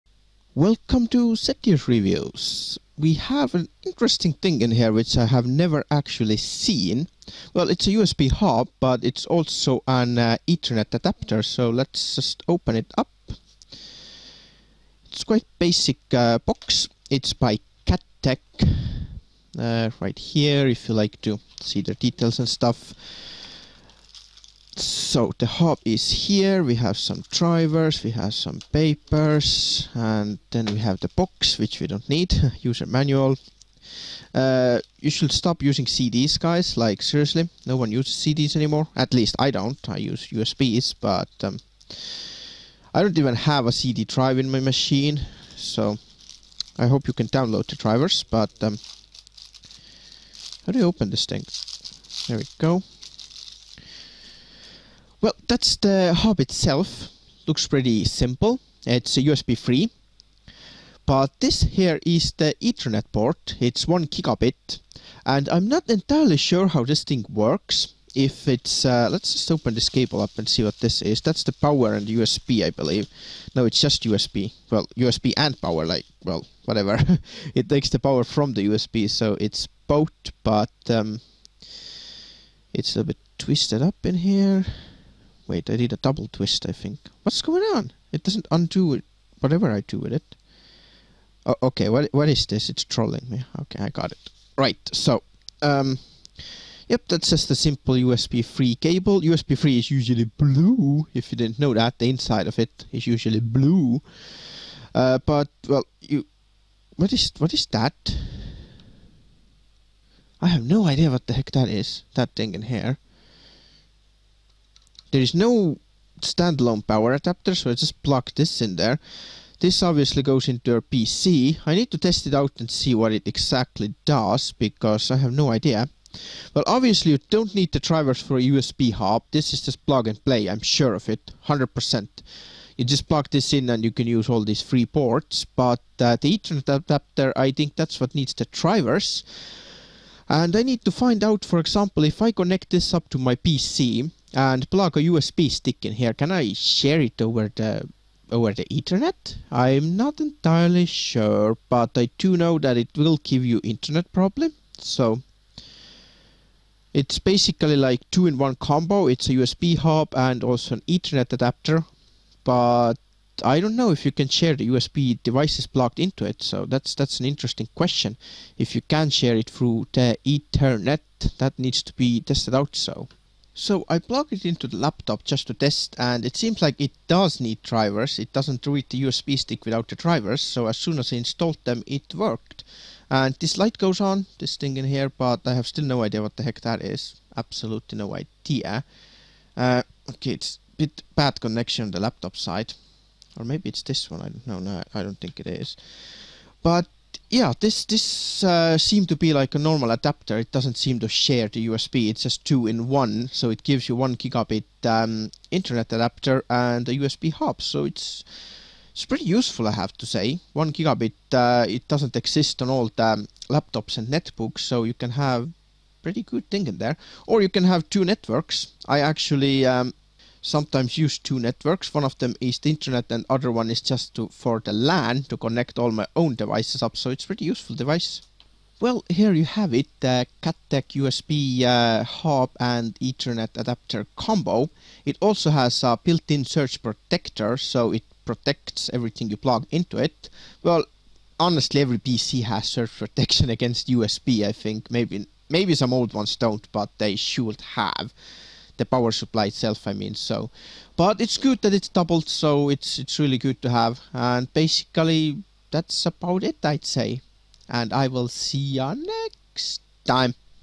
Cateck - USB 3.0 Hub + 1Gbit Ethernet Adapter Combo - REVIEW
Review of Cateck's new USB 3.0 Hub / 1gbit ethernet adapter combo. This device gives you a 1gbit ethernet port that you can use for internet or LAN + 3 extra USB 3.0 ports.
Just keep in mind that splitting 1 USB 3.0 port into 3, will STILL have the bandwidth of single USB 3.0 port (which is 5gbit).